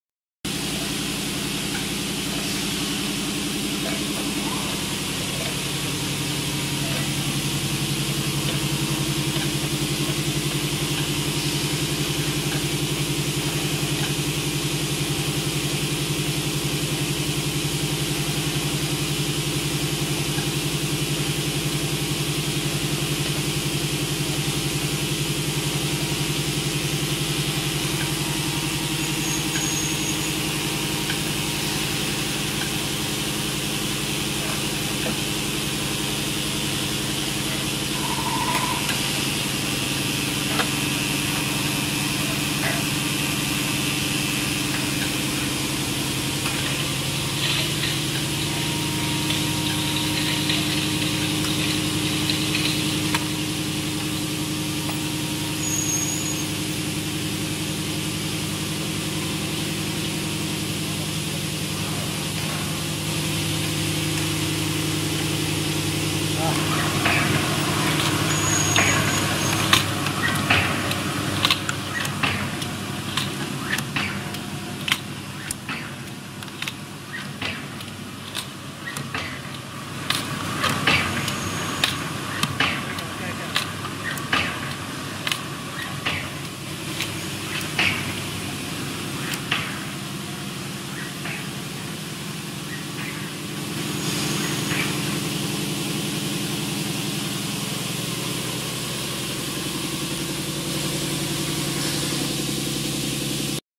Small bottle water juice soft drink filling machine
Our 3-in-1 water filling machine adopts bottle neck transmission technology to realize a fully automatic rinsing-filling-capping process. It is suitable for 200ml ~ 2000ml PET bottle beverage filling capping .
such as juice , drinking water , mineral water , flavored water , mineral water etc . even still water and sport water .We provide a complete 6-step process from factory design to after-sales support. Made by Reliable Machinery.